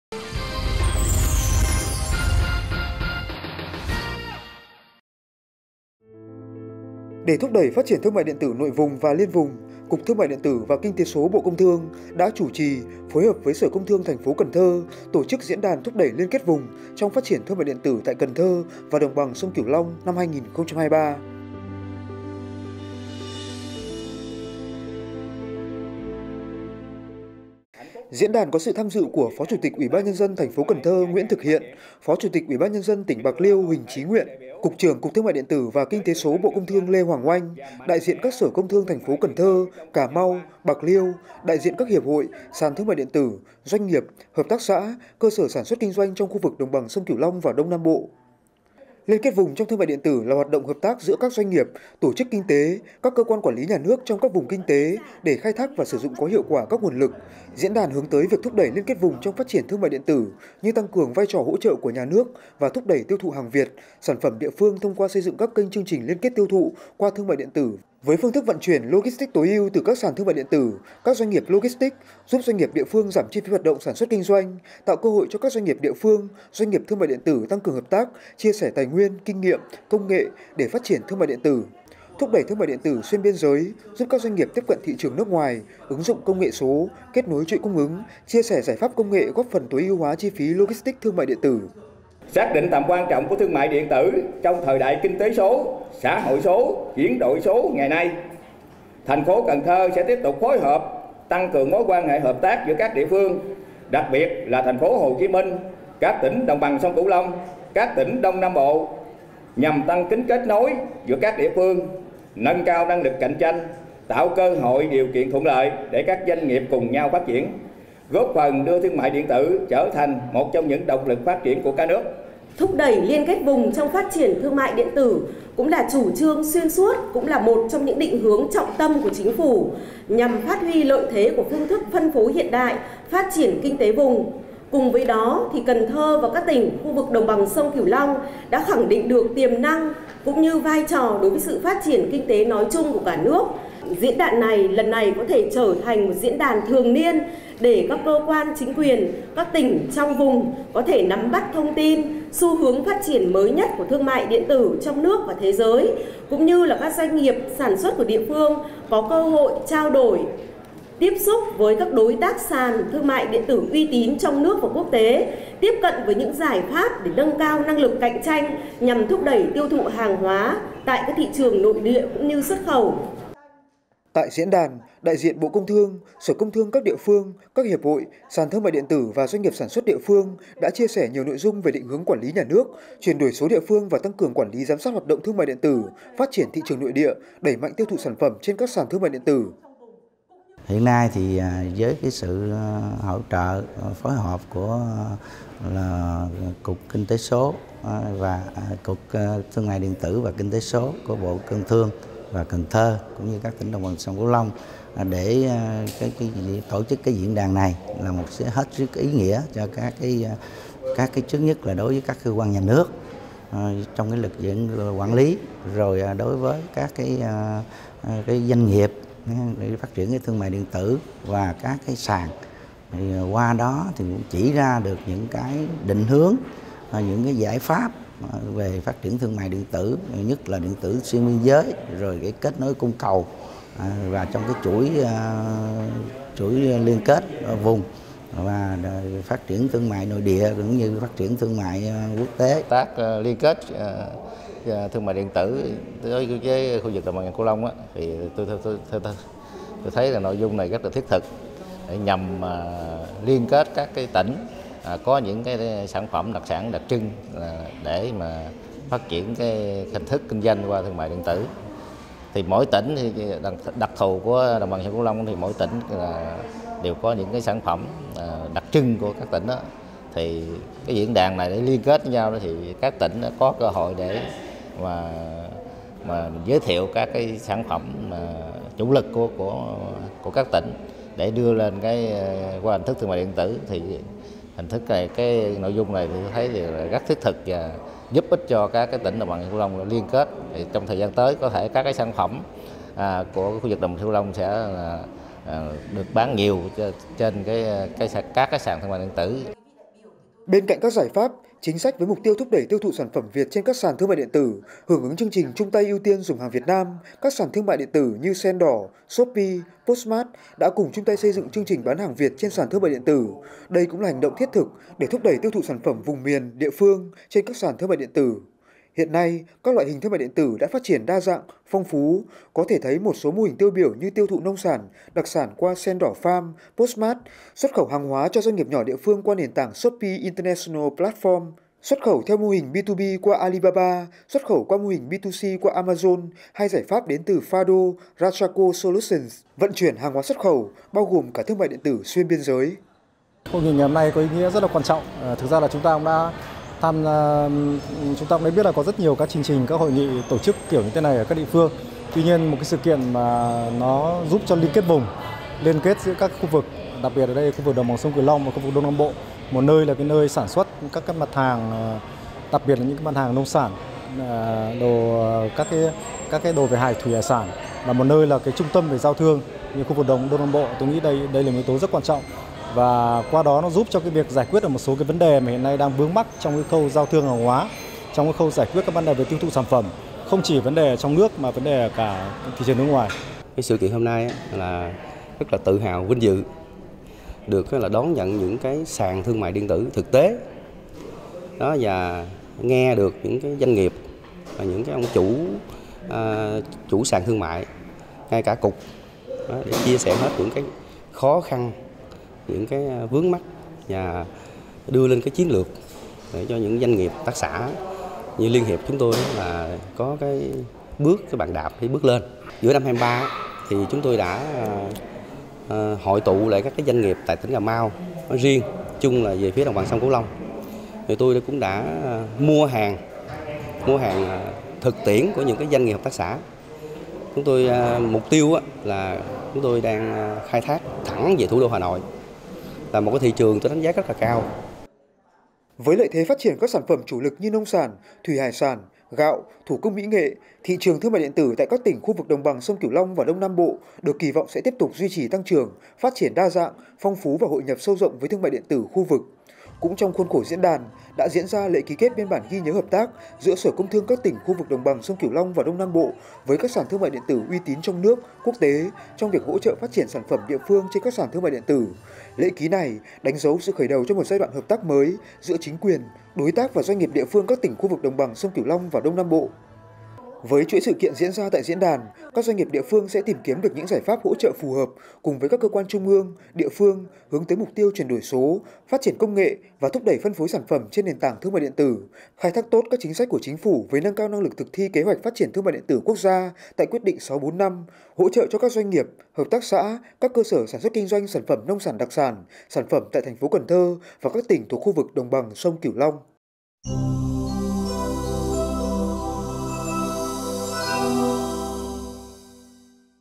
Thúc đẩy liên kết vùng trong phát triển thương mại điện tử
Để thúc đẩy phát triển thương mại điện tử nội vùng và liên vùng. Cục Thương mại điện tử và Kinh tế số -  Bộ Công Thương đã chủ trì, phối hợp với Sở Công Thương thành phố Cần Thơ tổ chức Diễn đàn thúc đẩy liên kết vùng trong phát triển thương mại điện tử tại Cần Thơ và đồng bằng sông Cửu Long năm 2023...